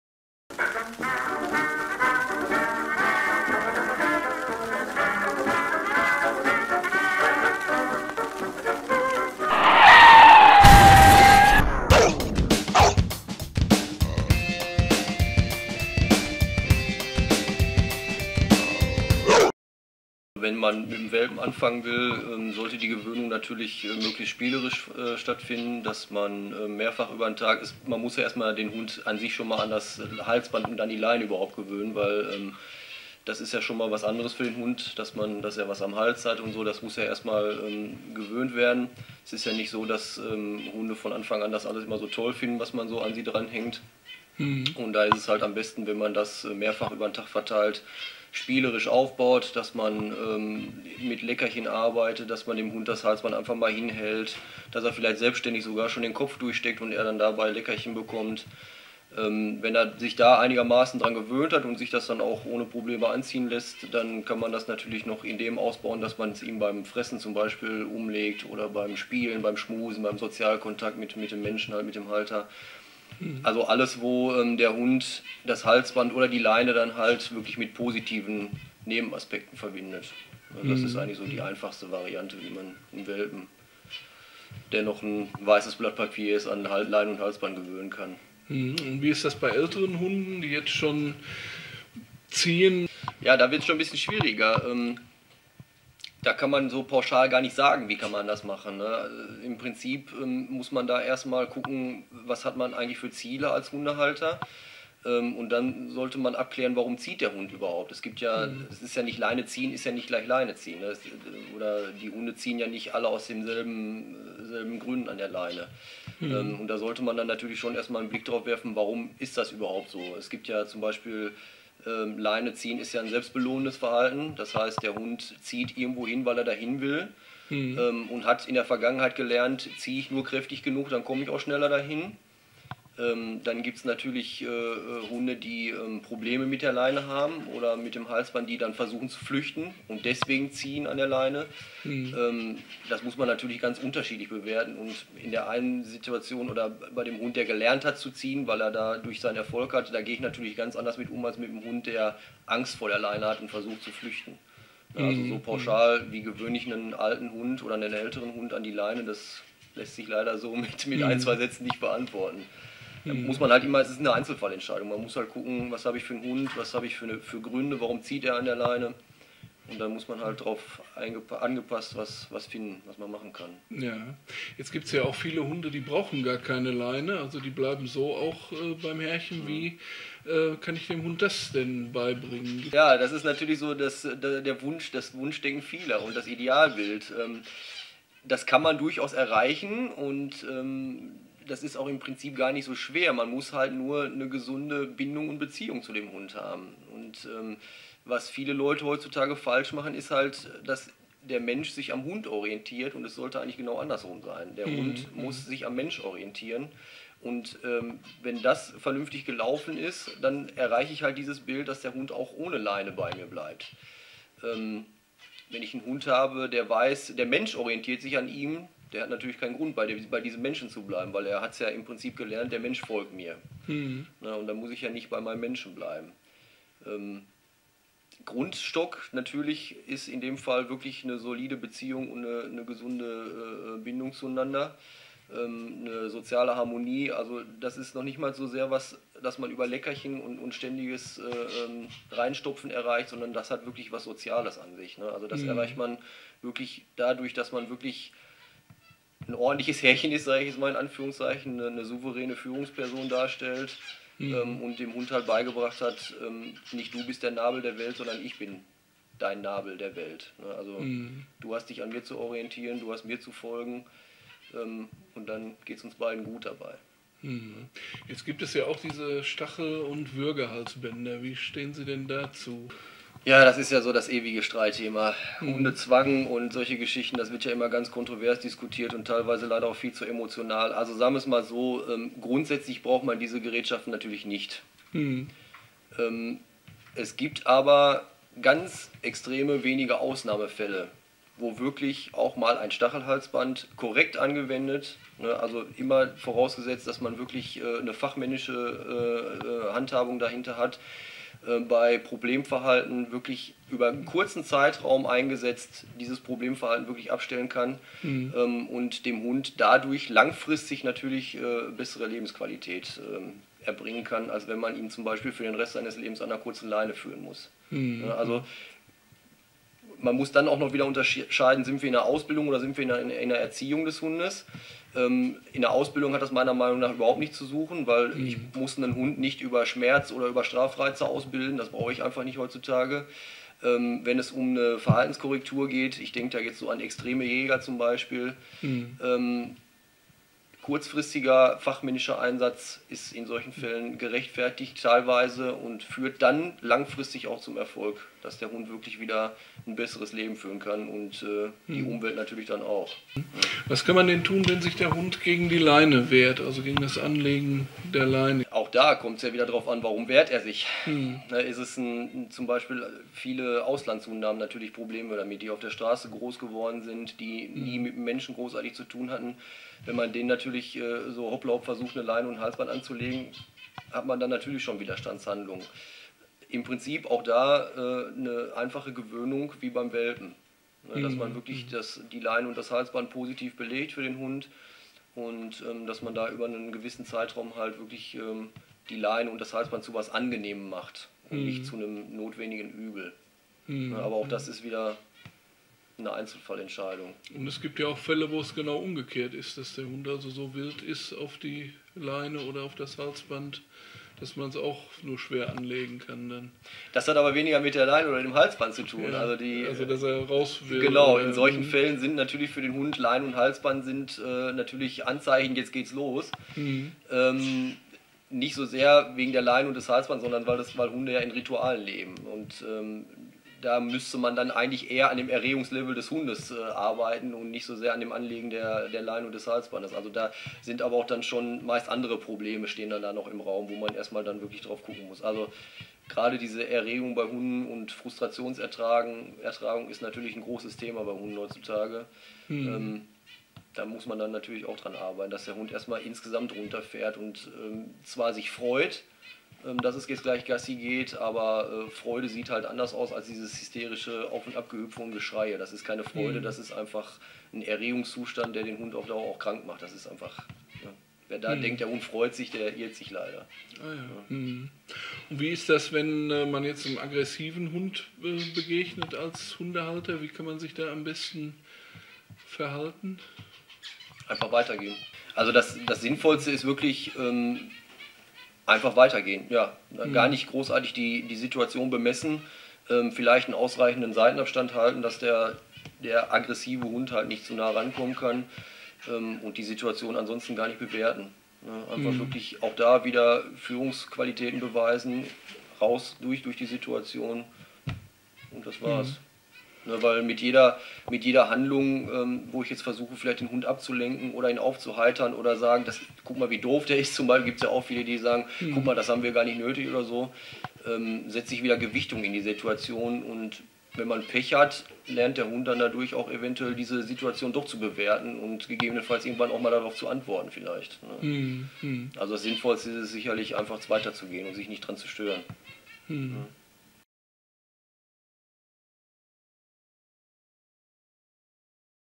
Hund an der Leine
zu Besuch beim Hunde-Experten Christoph Clemens der Infos gibt über das Gewöhnen des Hundes an die Leine, den richtigen Umgang mit der Leine und vieles mehr...